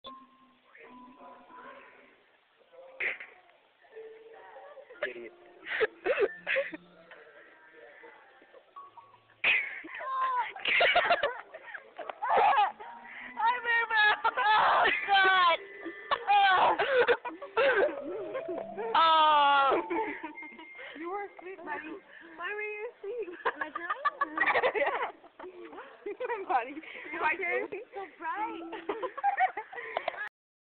We Woke Up Maribel
As we last saw Maribel, she was being documented by me, and telling sbout her oh so "cool" life. (not) well she fell asleep in the fine arts room and we woke her up by jumping on her